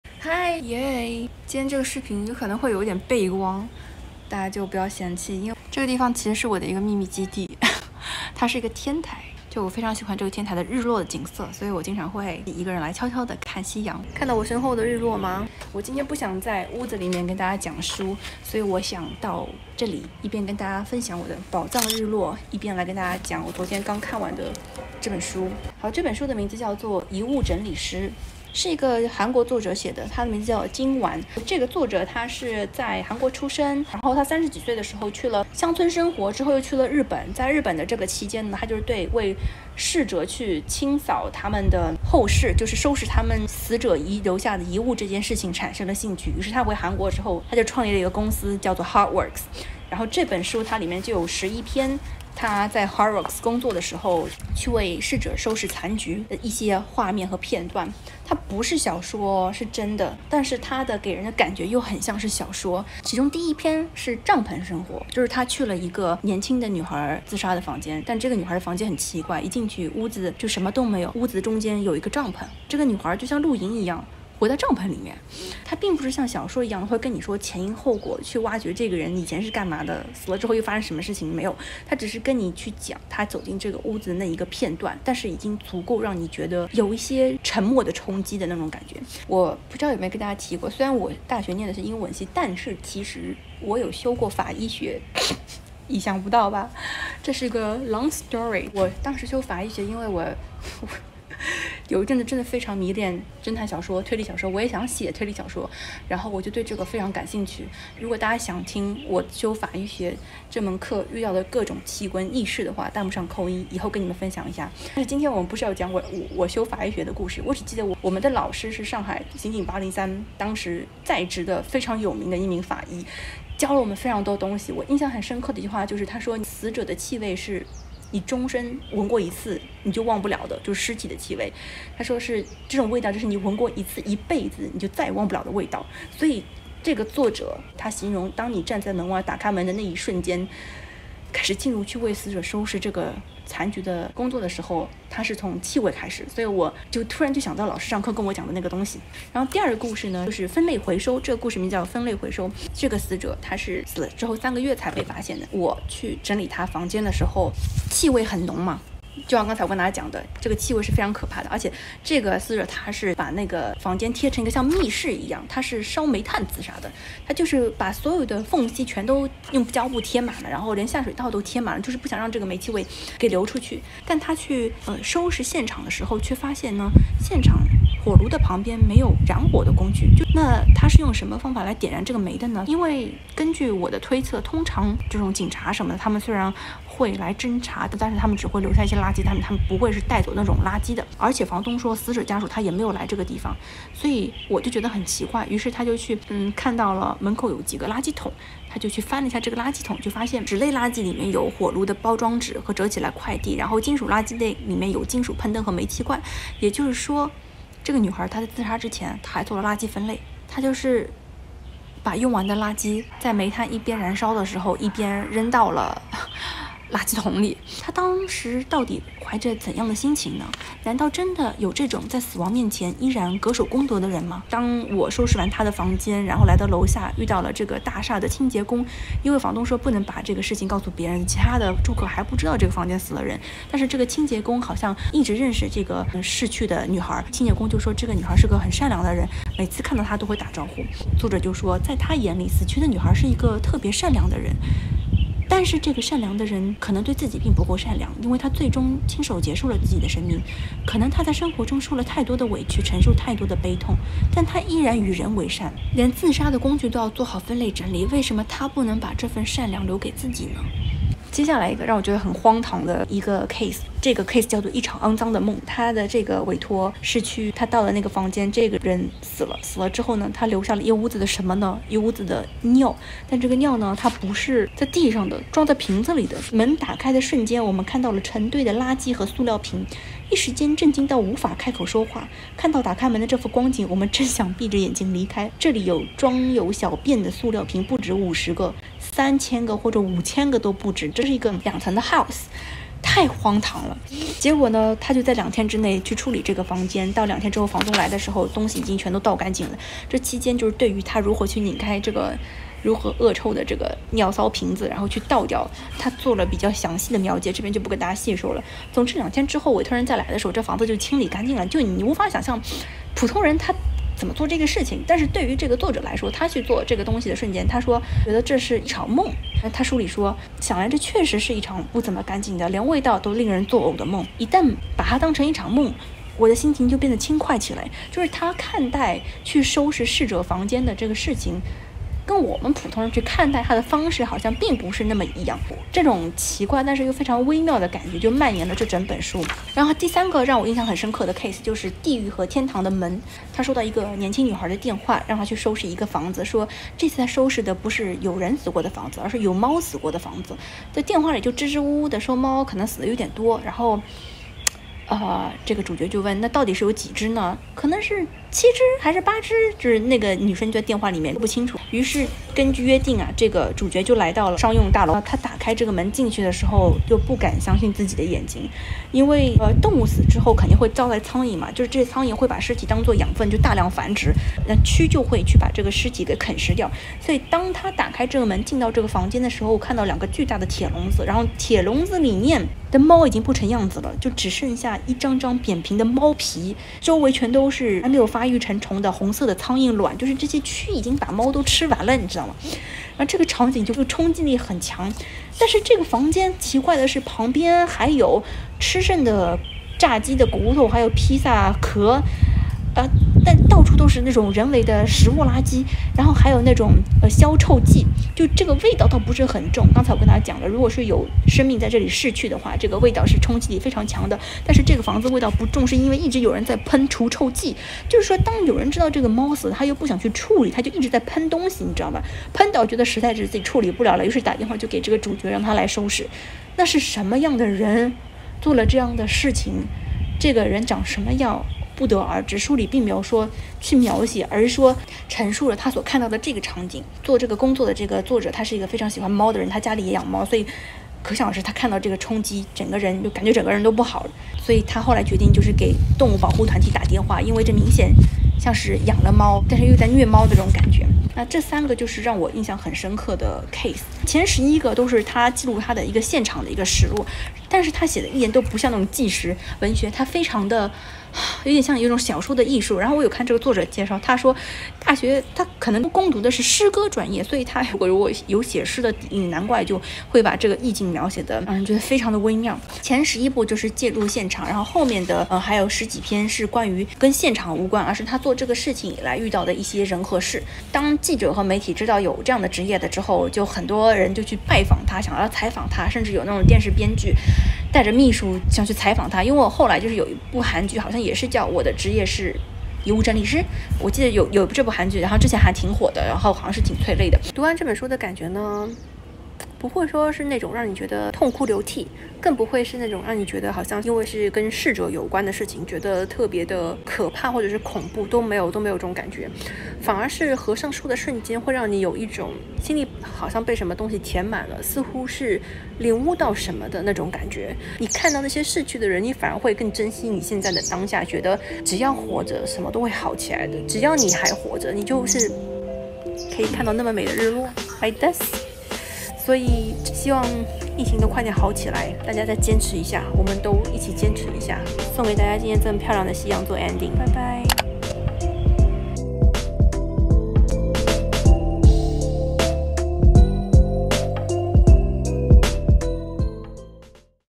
【慎入】韩国特殊职业《遗物整理师》，每一页都温柔又可怕 😭 / Puckio日韩文学系列
❤️【Book Info】
《遗物整理师》作者： 金完
“当有人独自死去，我的工作就开始了。”
本书作者在韩国从事着鲜为人知的特殊的整理工作，
专门负责整理逝者的现场。
从“为逝者清扫房间”这一工作过程中，思考生命以及生存的意义。

❤️【 关于 Puckio、
微博/B站/小红书 @小说家Puckio